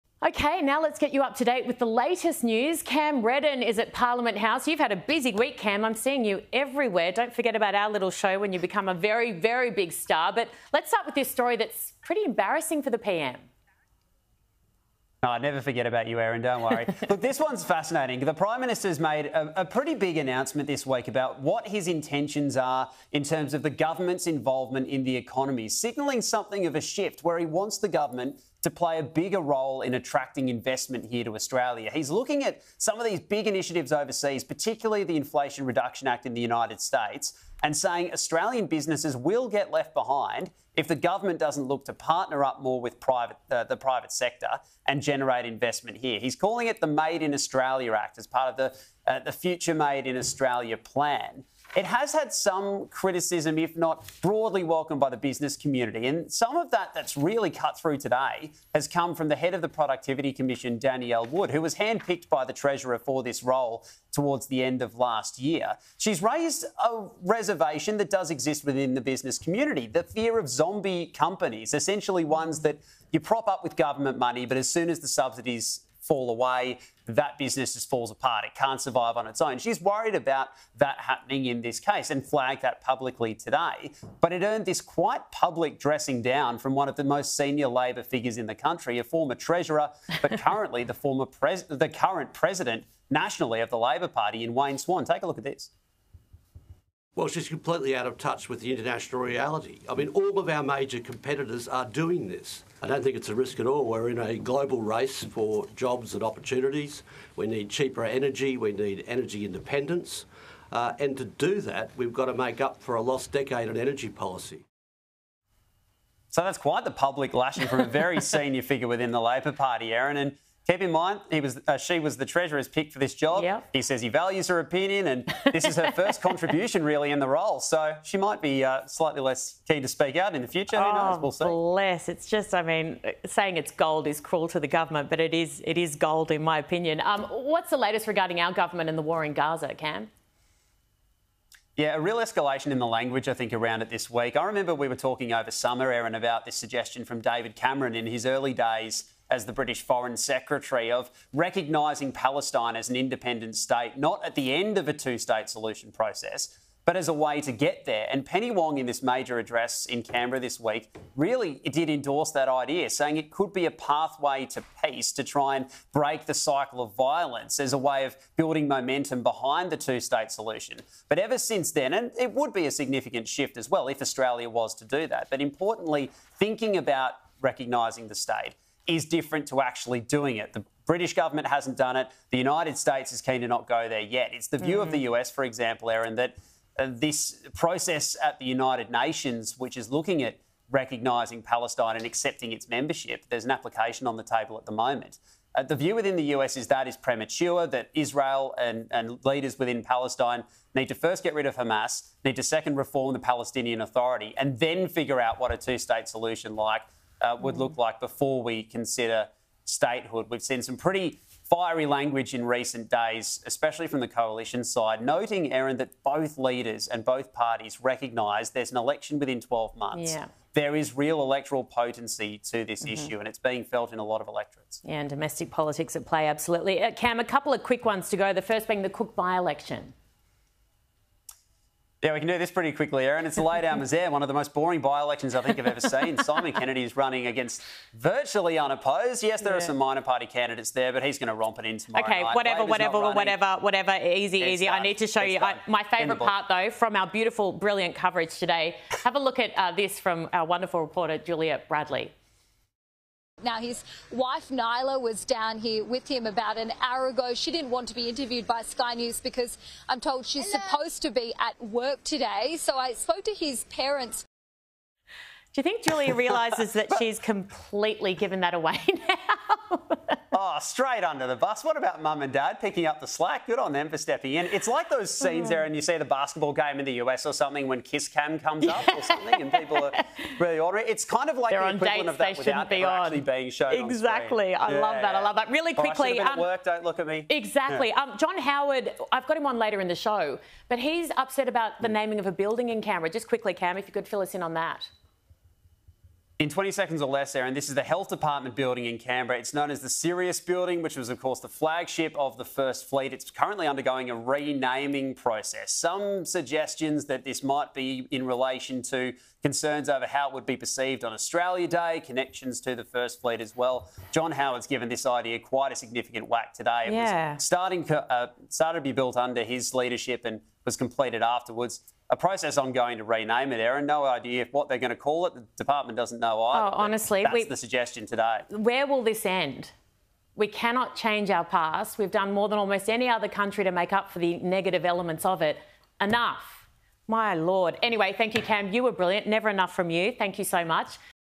‘Fear of zombie companies’: Productivity Commission CEO wary of ‘Made in Australia Act’
Productivity Commission CEO Danielle Wood has delivered a grave warning about Prime Minister Anthony Albanese’s ‘Made in Australia Act’.

“She’s raised a reservation that does exist within the business community,” Sky News Political Reporter Cam Reddin told host Erin Molan.

“The fear of zombie companies.

“Essentially, ones that you prop up with government money but as soon as the subsidies fall away that business just falls apart, it can’t survive on its own.

“She’s worried about that happening in this case and flagged that publicly.”